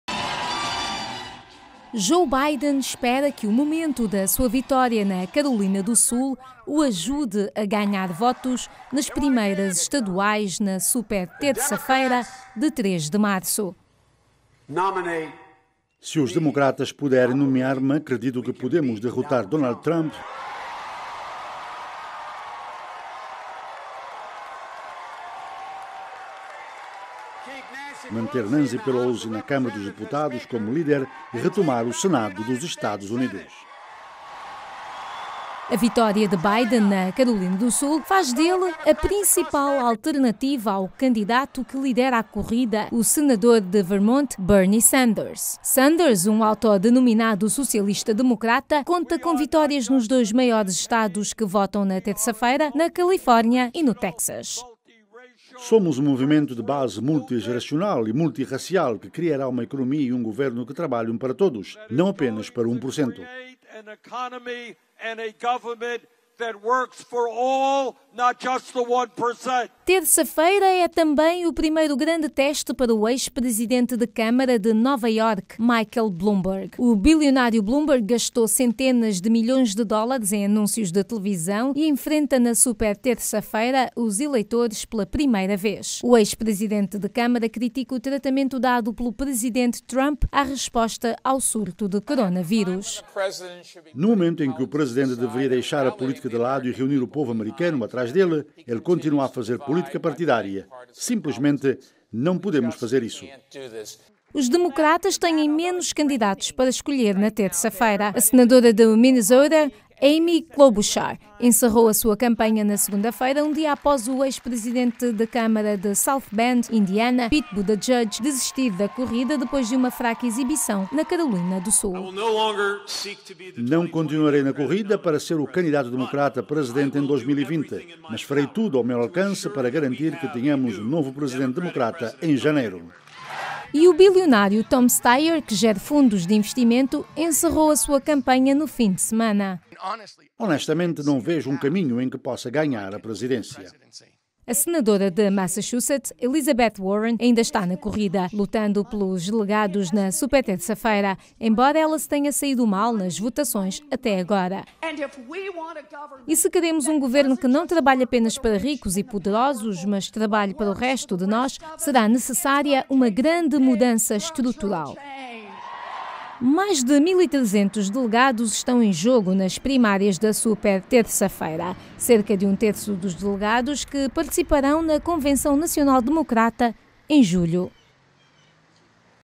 Super terça-feira: Em jogo mais de 1300 delegados
As alianças em mudança acontecem quando a batalha pela indicação presidencial do Partido Democrata atinge um novo nível de intensidade nesta terça-feira, 3 de março, quando 14 estados em todo o país realizam eleições primárias.